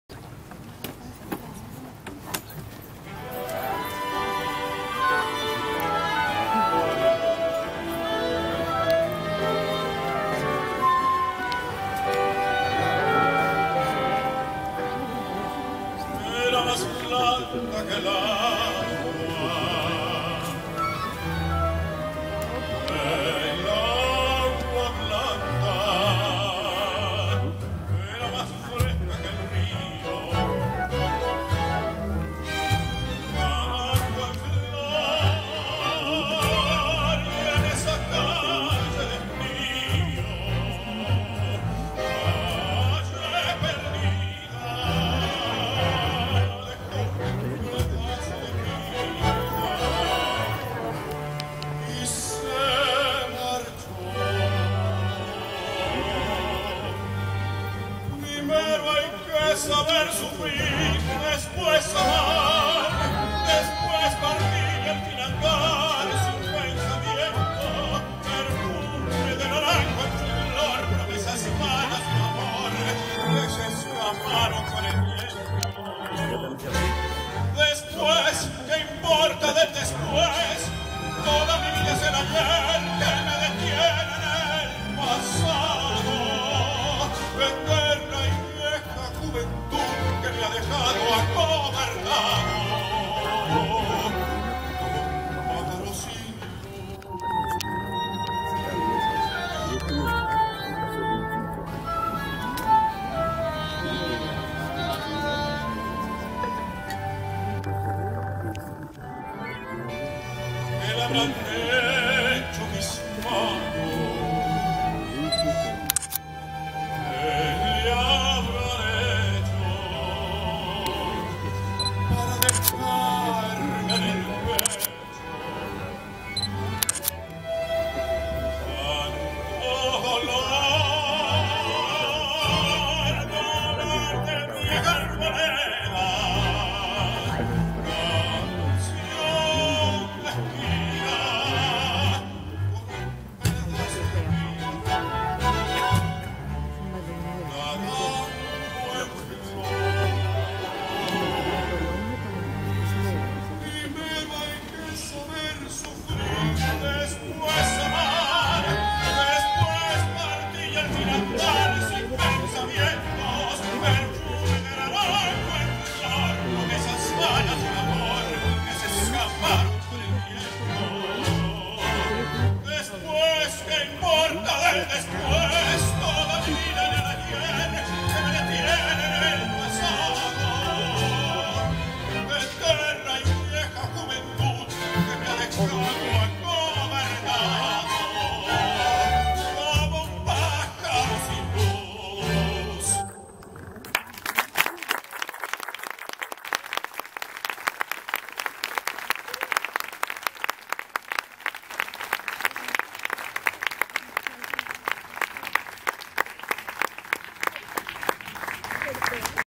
Noche de Tango y Milonga en la Casa de la Cultura de Tijuana con la OBC tango milonga SAM 3477
Noche de Tango y Milonga en la Casa de la Cultura de Tijuana con la Orquesta de Baja California.
Dir. Artístico y musical:
Mtro. Armando Pesqueira

Solista invitado:
Tenor Marco Antonio Labastida

Con la participación de:
Sin Fronteras Tijuana Tango y Tango Sensei

15 de Agosto 2019,
Escalinata Casa de la Cultura